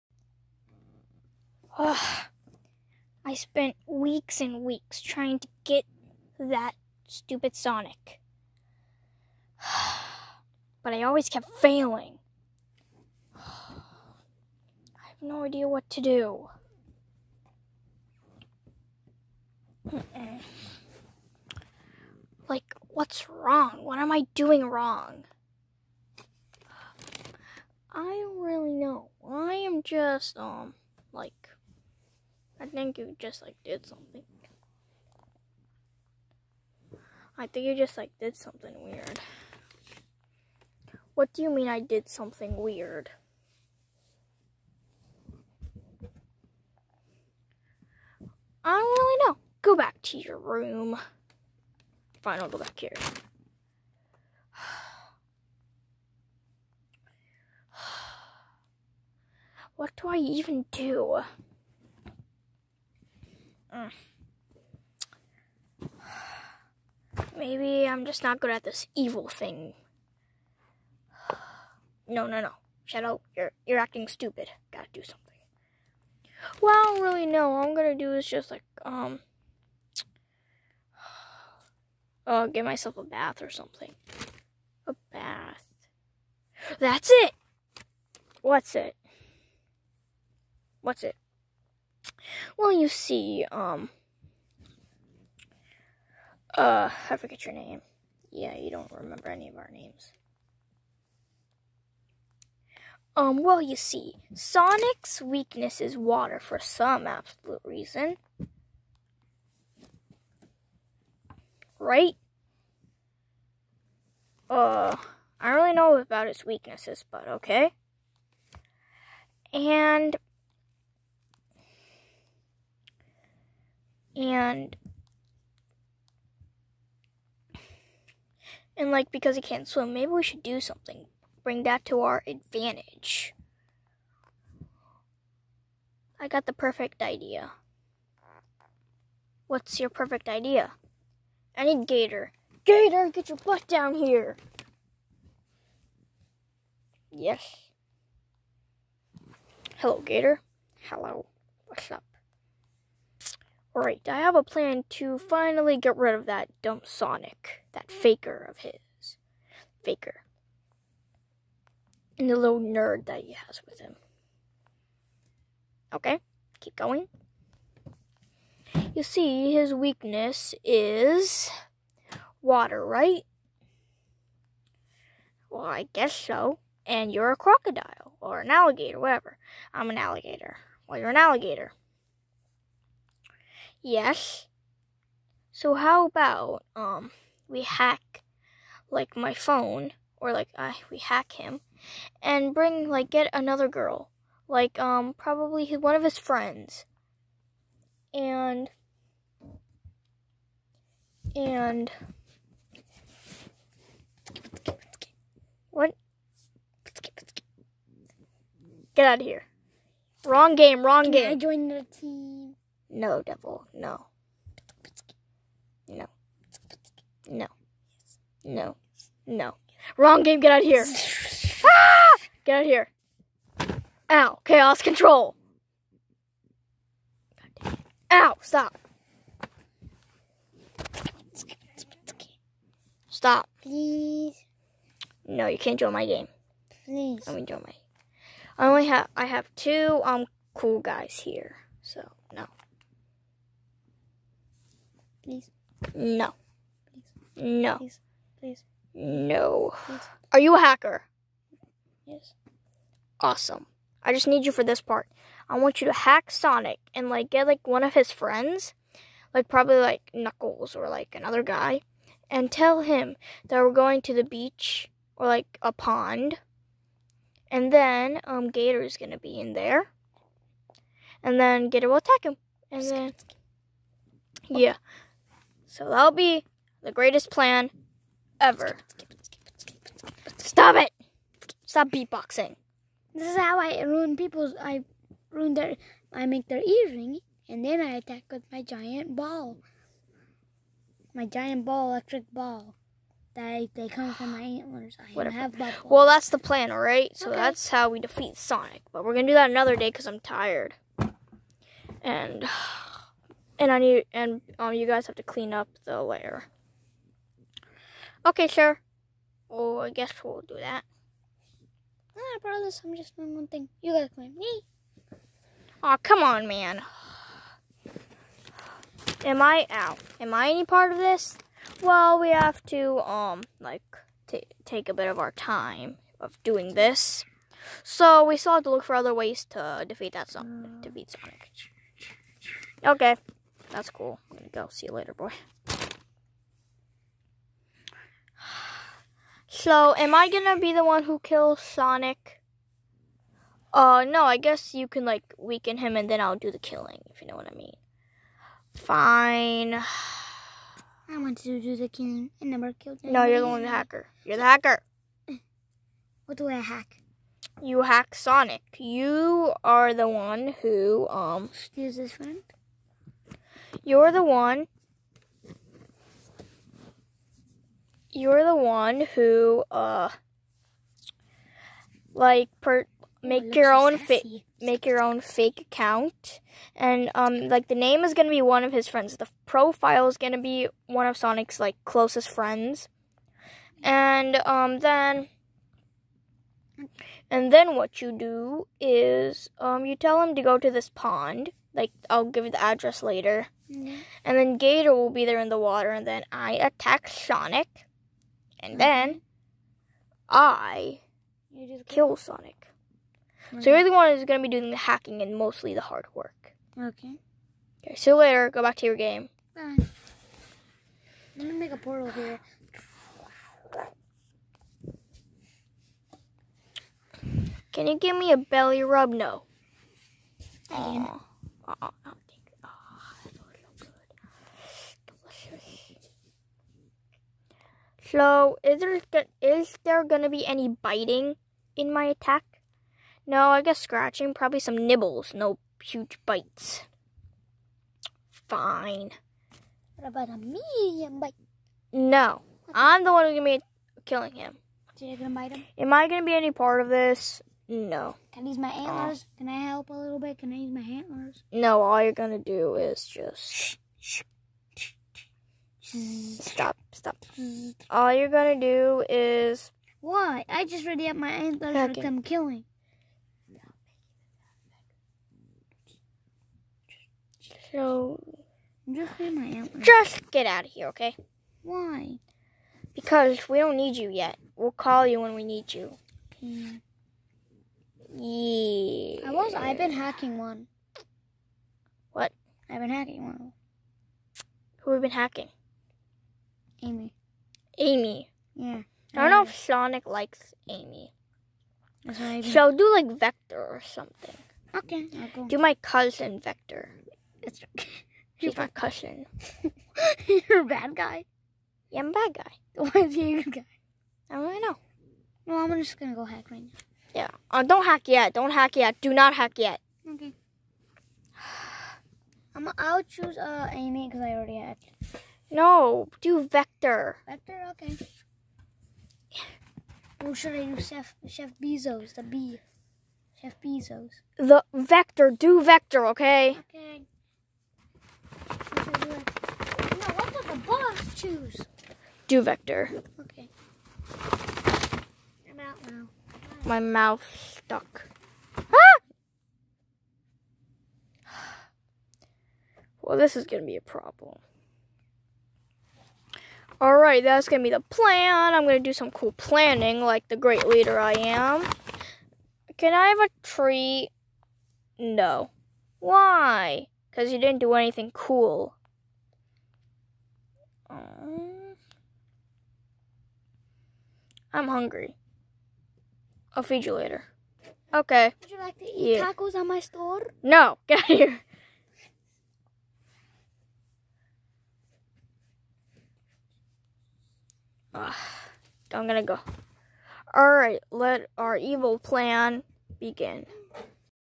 Shadow's evil plan (W.T.H.H.P. short)
Shadow made an evil plan to kill Sonic